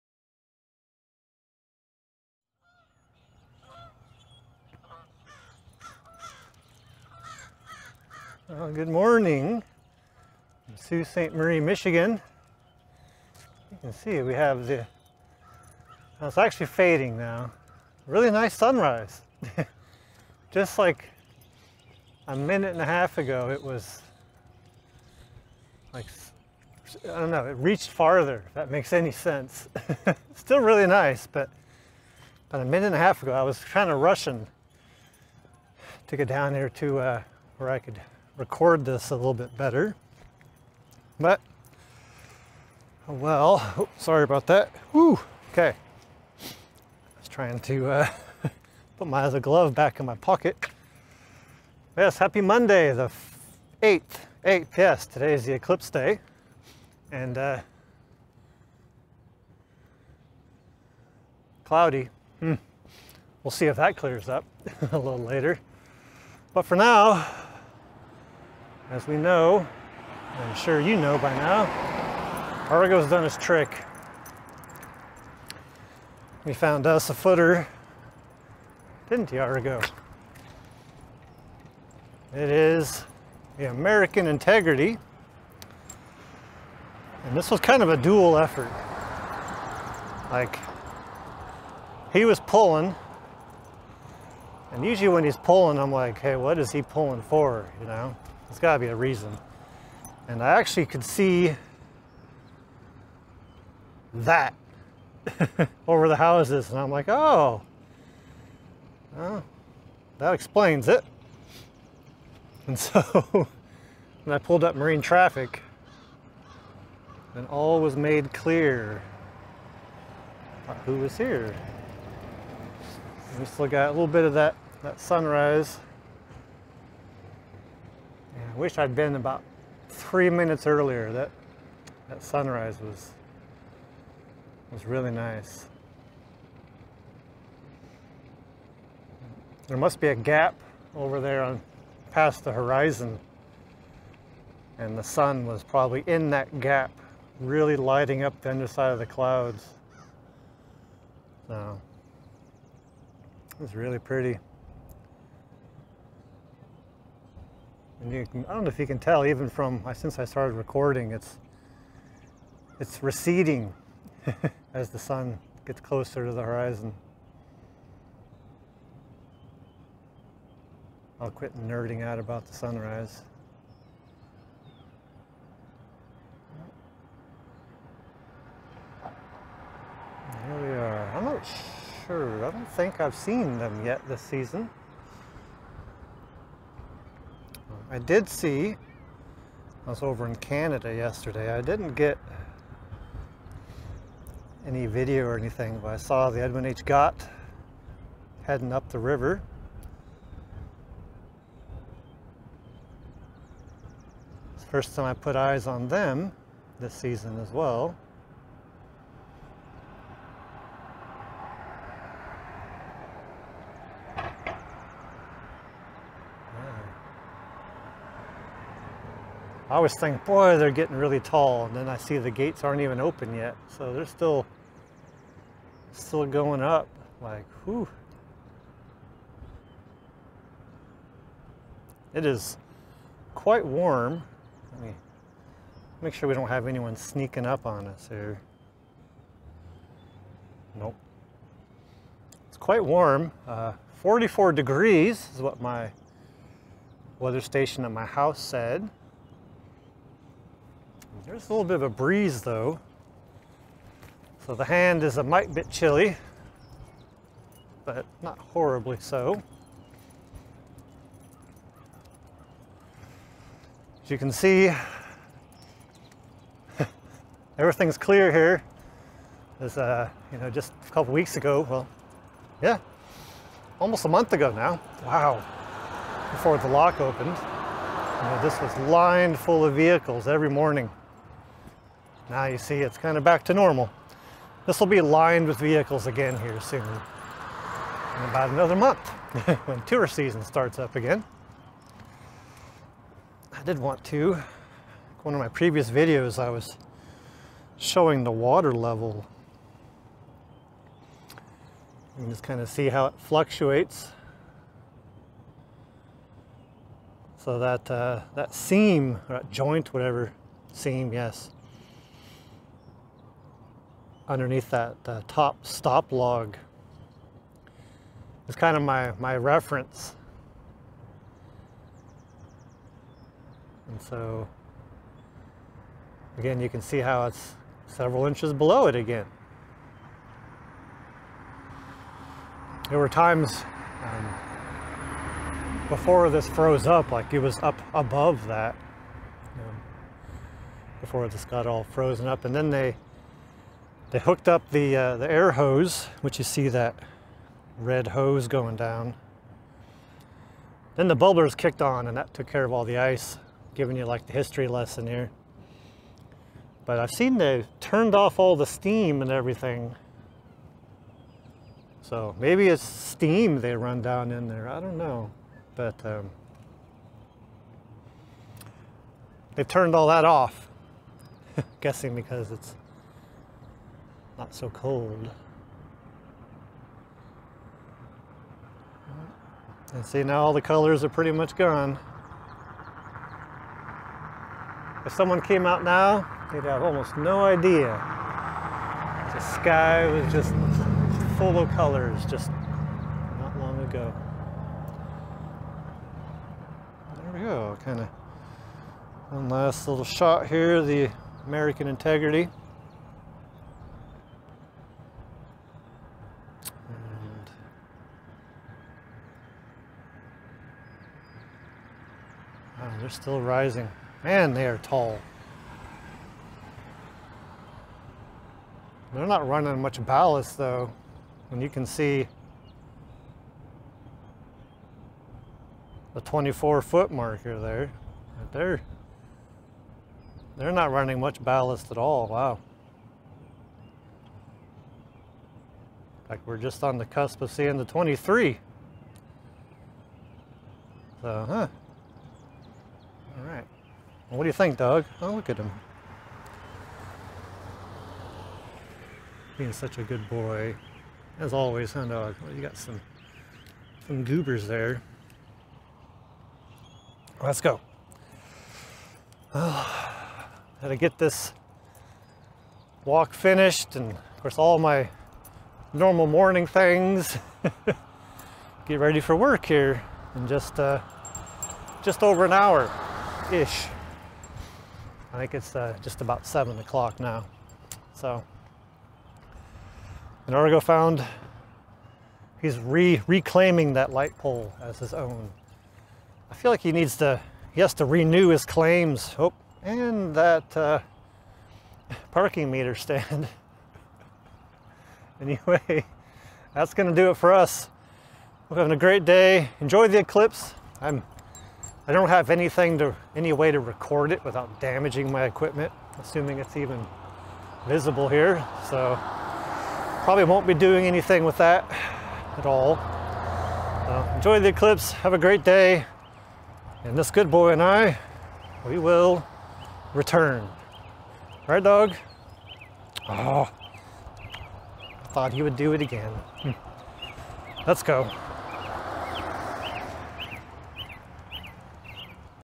Another great sunrise and another great footer
I was admiring the sunrise as Argo was eagerly pulling me towards the lock so I knew there must be a ship waiting for us.  Sure enough,  I spotted the American Integrity over the houses so I had a bit of warning before we got up to the Lock.